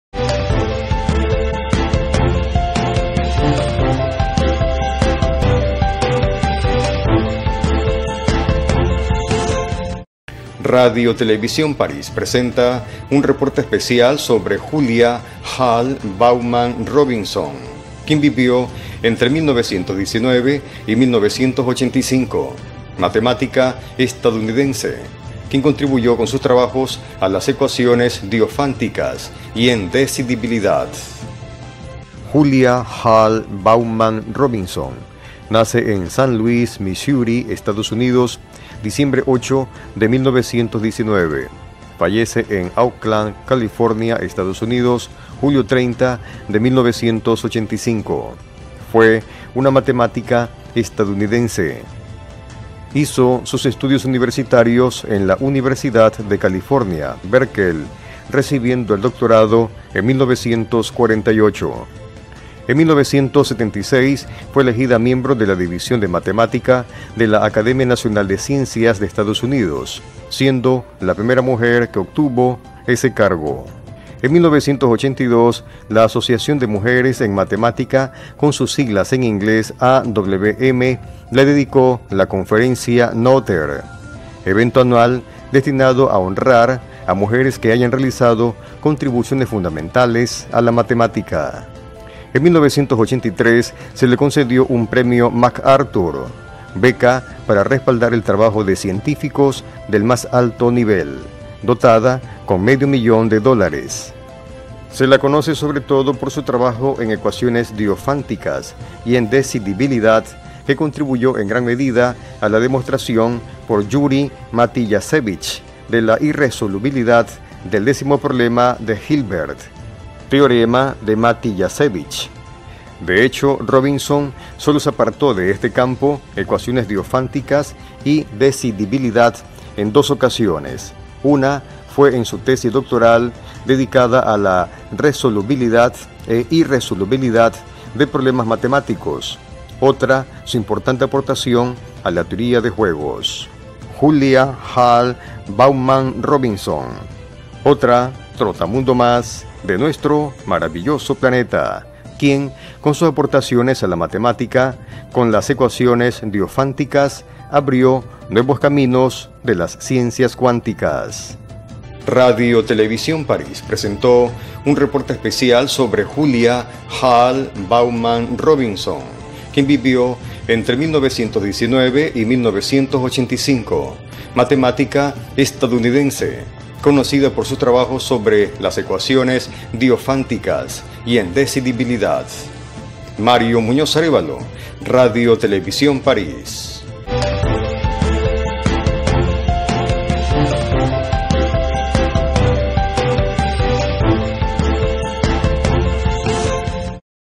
Homenaje a Julia Hall Bowman Robinson al 2017
Homenaje a Julia Hall Bowman Robinson, matemática estadounidense, quien aportó a las llamadas ecuaciones diofánticas, el día de hoy (Diciembre 08 del 2.017), se recuerda un año más de su natalicio, nota emitida en Radio y TV París de Vinces.